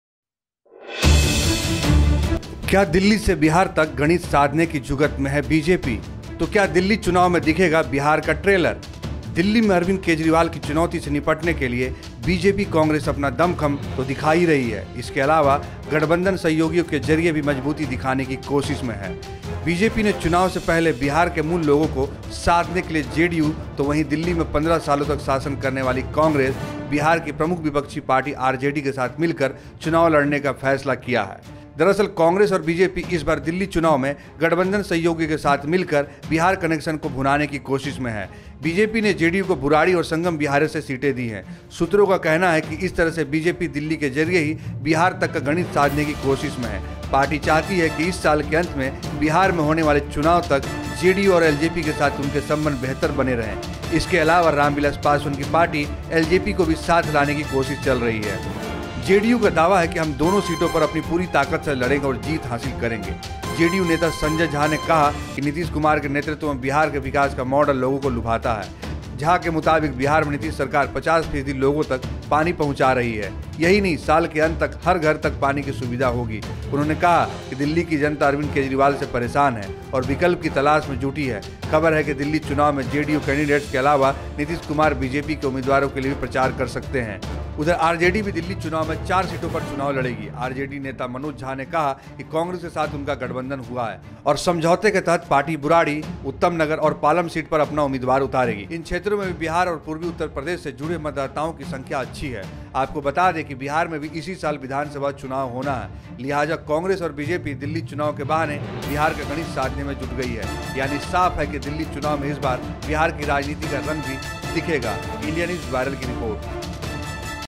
क्या दिल्ली से बिहार तक गणित साधने की जुगत में है बीजेपी ? INDIA NEWS VIRAL
क्या दिल्ली से बिहार तक गणित साधने की जुगत में है बीजेपी ? तो क्या दिल्ली चुनाव में दिखेगा बिहार का ट्रेलर ? जानिए रिपोर्ट में